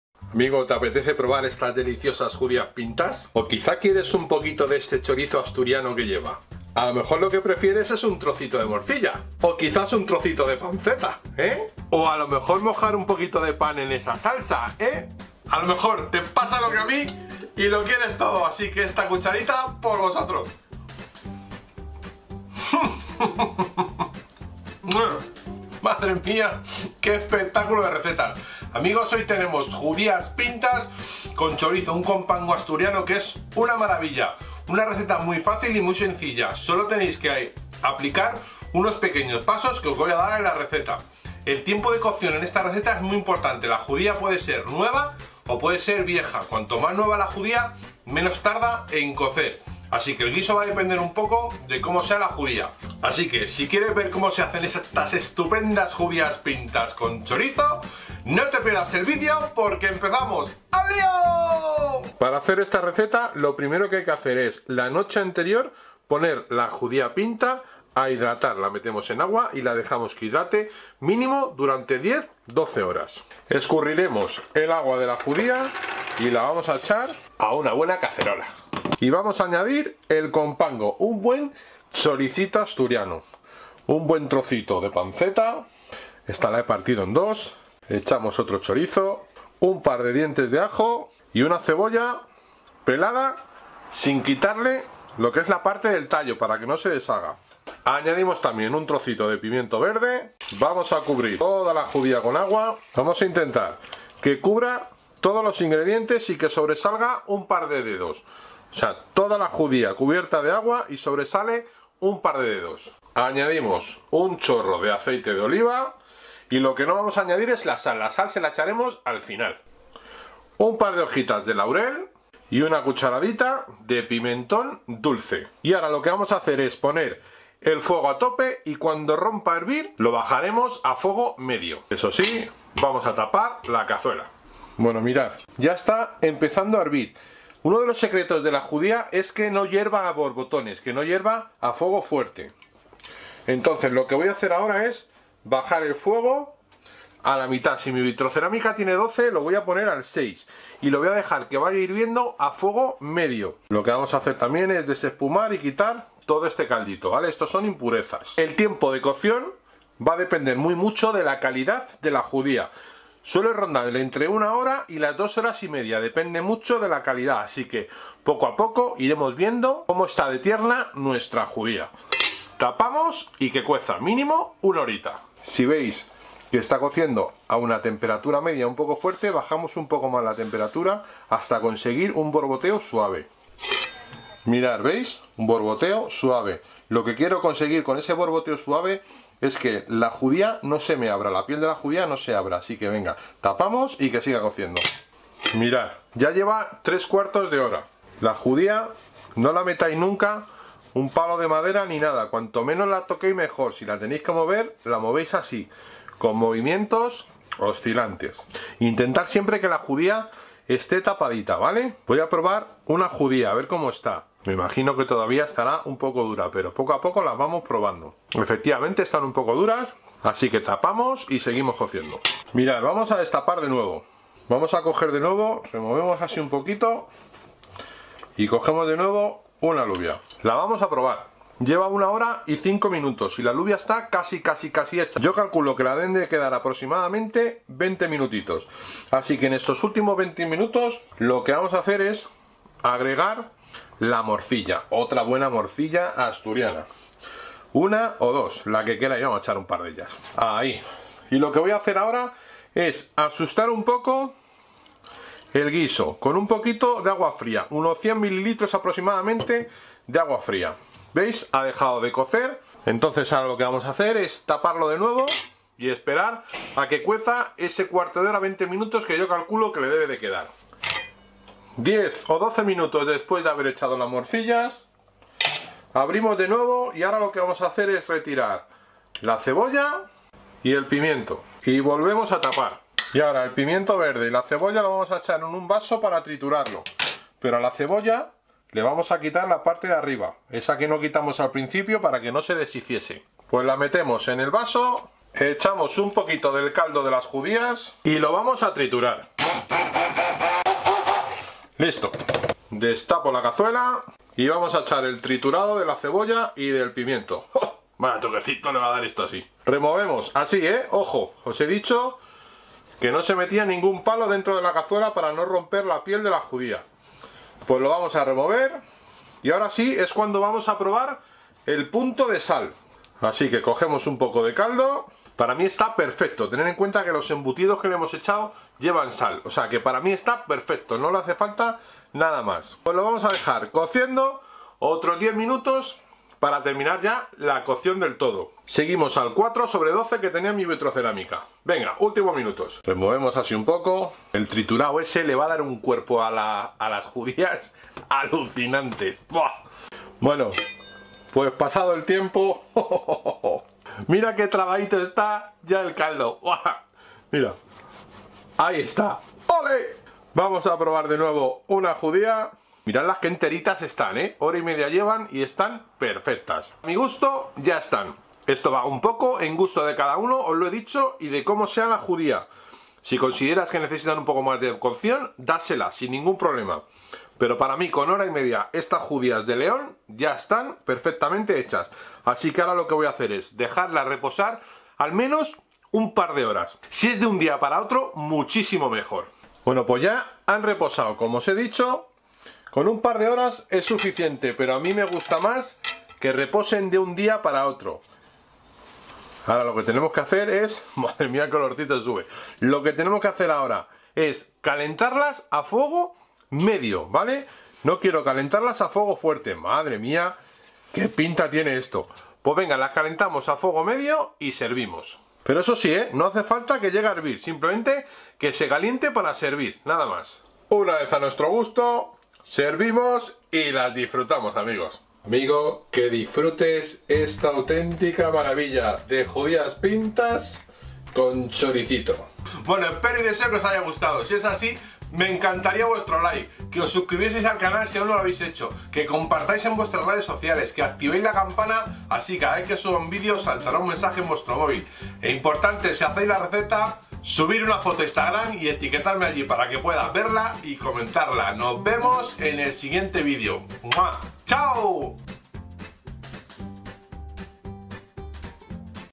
Judias pintas con chorizo ¡FACILES Y ESPECTACULARES!
Judias pintas o alubias pintas, una receta muy fácil, de la cocina tradicional española, realizada con productos de león ( la judía) o de asturias ( el compango). te la recomiendo, esta es una receta que realizada a fuego lento y con buenos productos es un exito seguro, COMPRUEBALO

INGREDIENTES PARA 6
500 GR DE JUDIAS PINTAS
1.2 L DE AGUA + 100 ML EXTRAS
2 CHORIZOS
2 MORCILLAS
200 GR DE PANCETA ADOBADA
1 CEBOLLA
MEDIO PIMENTO VERDE
2 DIENTES DE AJO
2 HOJAS DE LAUREL
40 ML DE ACEITE DE OLIVA
1/2 CUCHARADITA DE PIMENTON DULCE